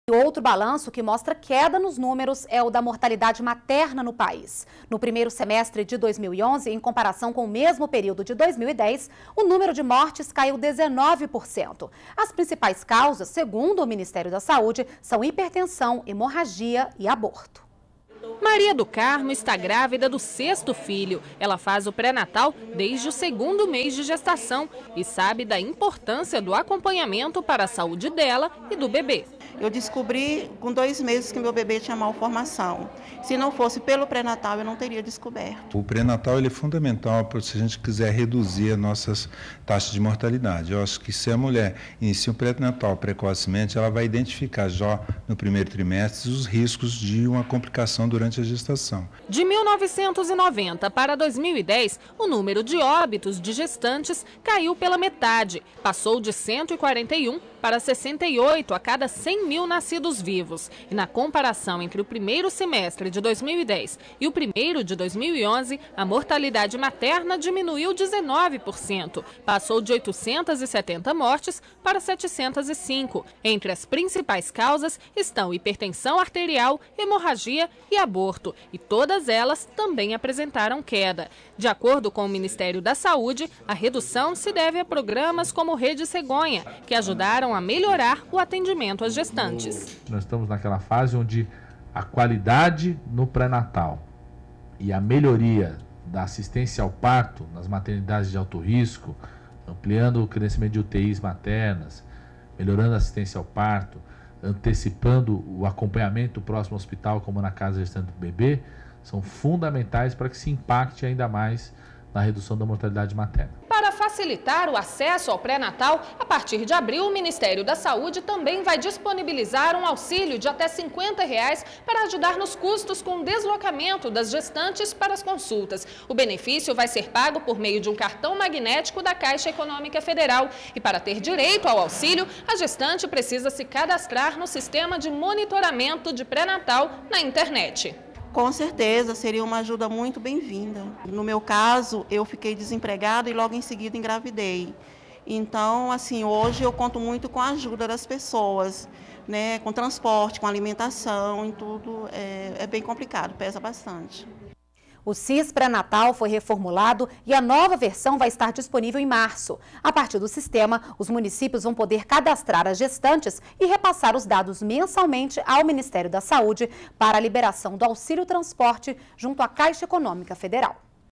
Cai 19% número de mortalidade materna no 1º semestre de 2011 em relação a 2010
NBR Notícias - 23.02.12: Outro balanço que mostra queda nos números é o da mortalidade materna no país. No 1° semestre de 2011, em comparação com o mesmo período de 2010, o número de mortes caiu 19%. As principais causas, segundo o Ministério da Saúde, são hipertensão, hemorragia e aborto. O Sisprenatal foi reformulado e a nova versão vai estar disponível em março. A partir do sistema, os municípios vão poder cadastrar as gestantes e repassar os dados, mensalmente, ao Ministério da Saúde para a liberação do auxílio transporte junto à Caixa Econômica Federal.